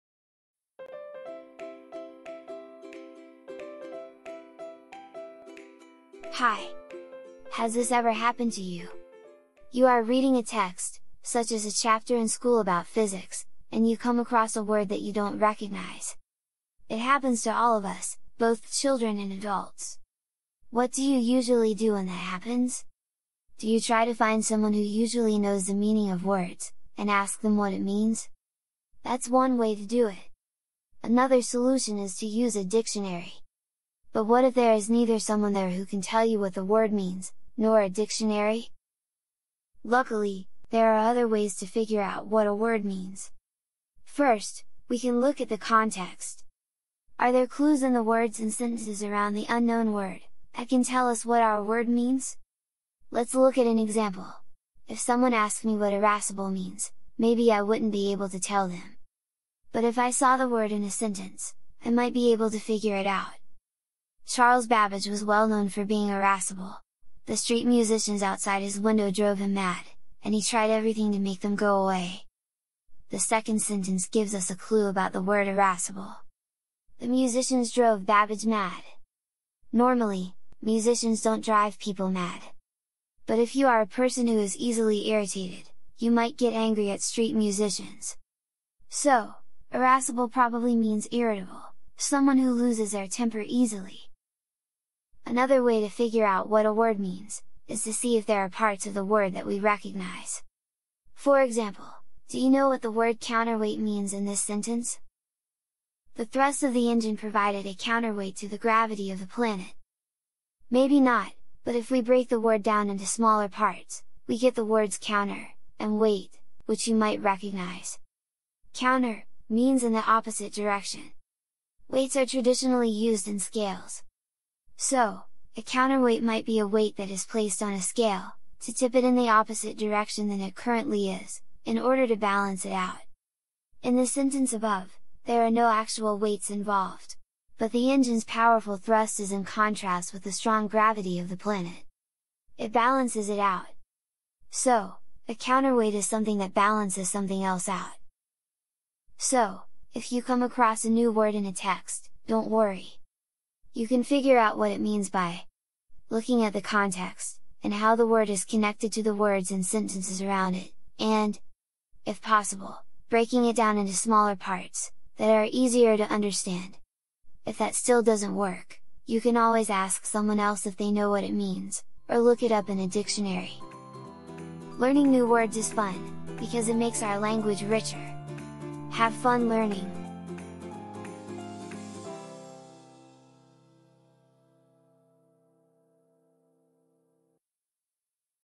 Figuring out the meaning of words in informational texts
What do you do if you come across a word in a scientific text that you don't recognize? There are different ways to find out what a word means, and in this instructional video Owen presents some of them.
This video can be used to work with CCSS.ELA-Literacy.CCRA.R.4: "Interpret words and phrases as they are used in a text, including determining technical, connotative, and figurative meanings, and analyze how specific word choices shape meaning or tone."
It is part of a broader lesson plan on determining the meaning of academic words. To access the lesson plan and other fun, educational resources, please visit Snowflake.live.

Video music byREDproductions from Pixabay.